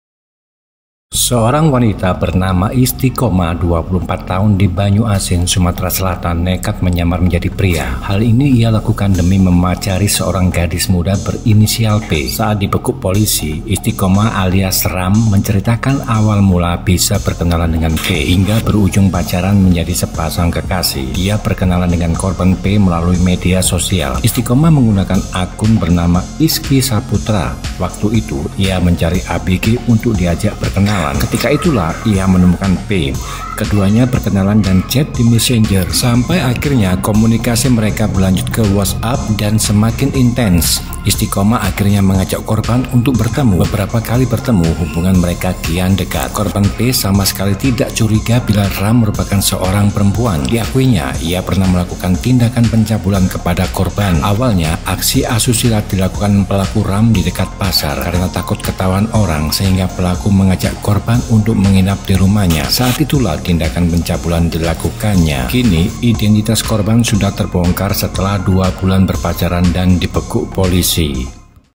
Seorang Wanita Menyamar Jadi Pria untuk Pacari Gadis ABG di Banyuasin, Kenal Lewat Medsos.
Seorang wanita bernama Istiqomah (24) di Banyuasin, Sumatera
Selatan nekat menyamar menjadi pria.

Hal ini ia lakukan demi memacari seorang gadis muda berinisial P.

Saat dibekuk polisi, Istiqomah alias Raam menceritakan awal mula
bisa berkenalan dengan P.

Hingga berujung pacaran menjadi sepasang kekasih.

Istiqomah menggunakan akun bernama Isqi Saputra.

Waktu itu, ia mencari ABG untuk diajak berkenalan.

Ketika itulah, ia menemukan P.

Keduanya berkenalan dan chat di Messenger.

Sampai akhirnya komunikasi mereka berlanjut ke WhatsApp dan
semakin intens.

Istiqomah akhirnya mengajak korban untuk bertemu.

Beberapa kali bertemu, hubungan mereka kian dekat.

Korban P sama sekali tidak curiga bila Raam merupakan seorang
perempuan.

Diakuinya, ia pernah melakukan tindakan pencabulan kepada
korban.

Awalnya aksi asusila dilakukan pelaku Raam di dekat pasar.

Karena takut ketahuan orang, sehingga pelaku mengajak korban
untuk menginap di rumahnya.

Saat itulah, tindakan pencabulan dilakukannya.

Kini, identitas korban sudah terbongkar setelah dua bulan
berpacaran dan dibekuk polisi.(*)

(ren)